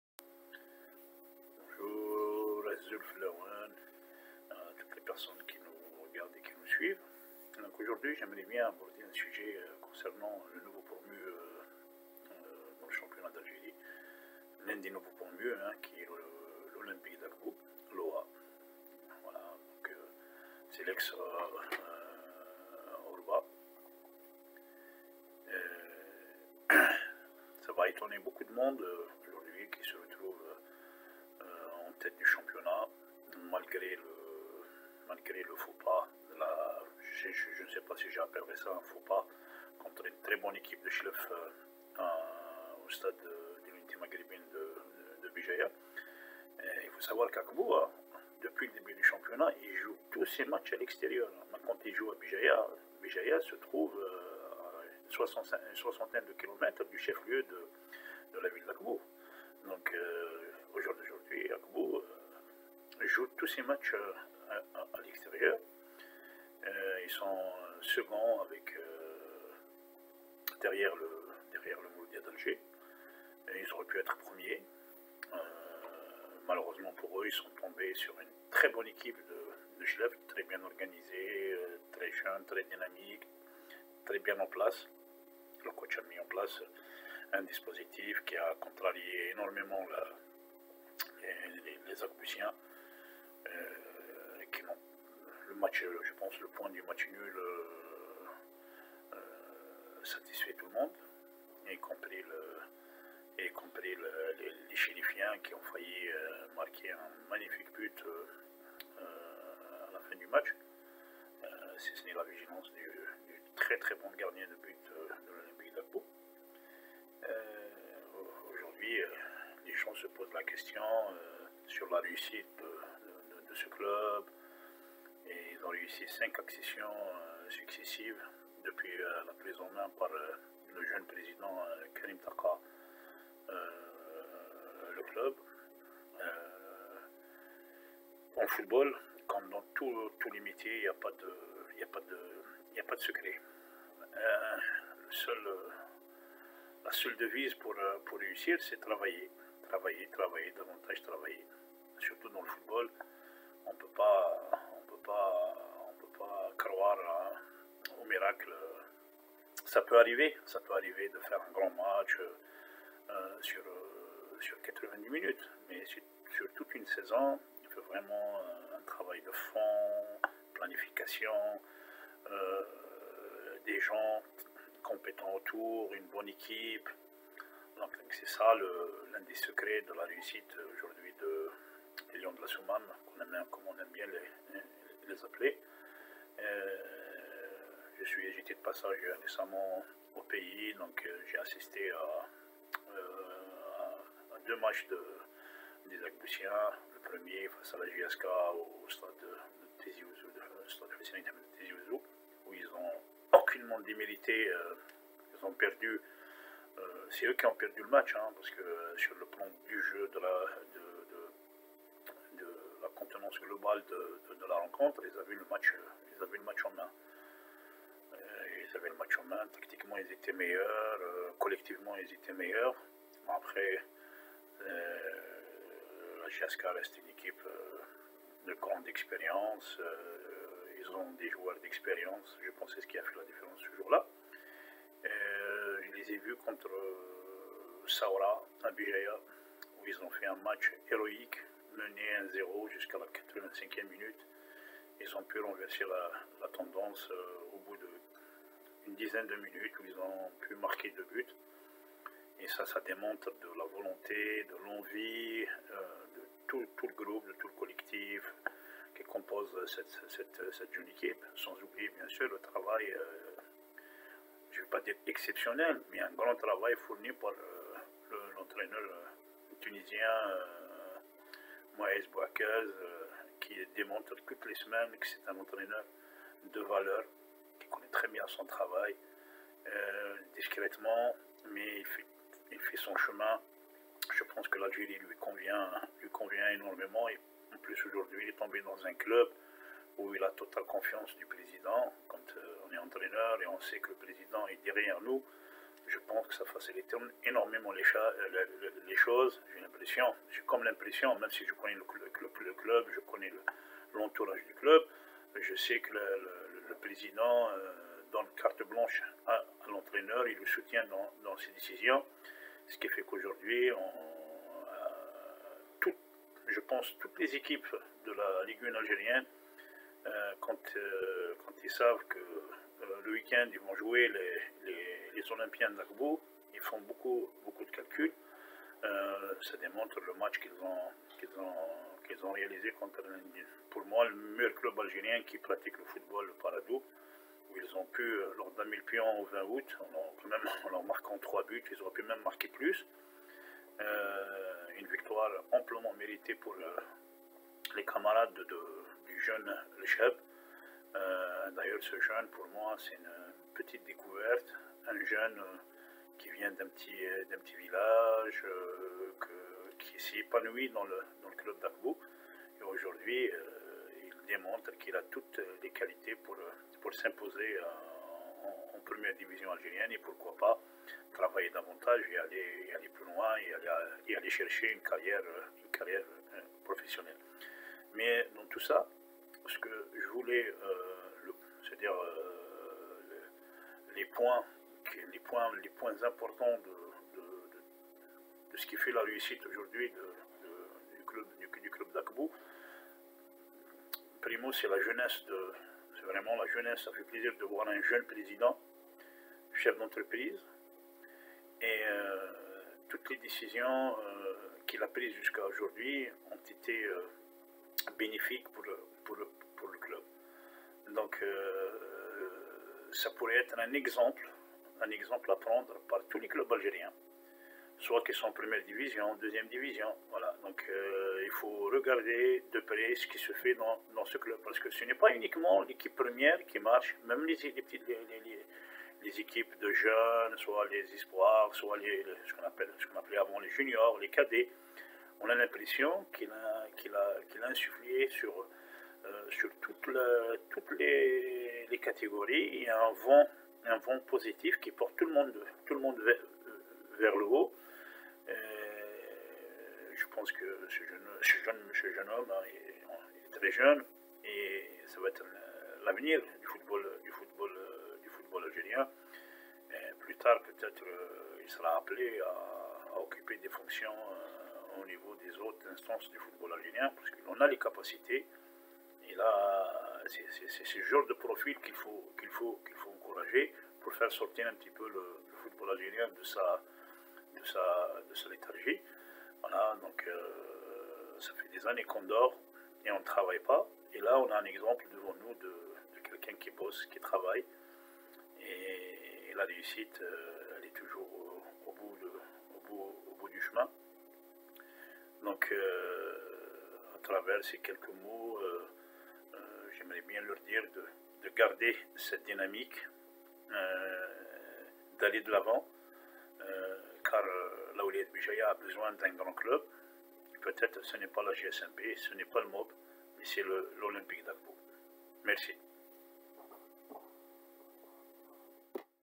L'Olympique d'Akbou, un club vraiment ambitieux.
l'Olympique d'Akbou pourrait créer la surprise cette saison. c'est un club bien structuré et surtout bien géré par un président jeune et ambitieux. Il a su choisir l'homme qu'il faut pour diriger un groupe de jeunes joueurs pétris de qualités